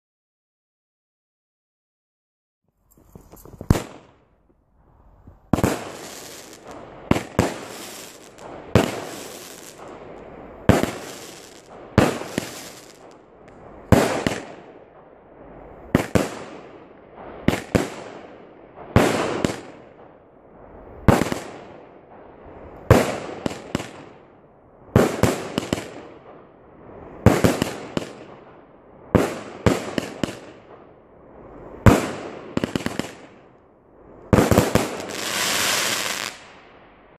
Burning Angel - Zena Vuurwerk 3131 [OFFICIAL VIDEO]
Burning Angel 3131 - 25 schots cake uit de Impress Collection van Zena Vuurwerk.

De Burning Angel is een 25 schots cake uit de nieuwe lijn van Zena Vuurwerk. Speciaal vuurwerk op een speciale manier gefabriceerd waardoor het zeer goedkoop aangeboden kan worden.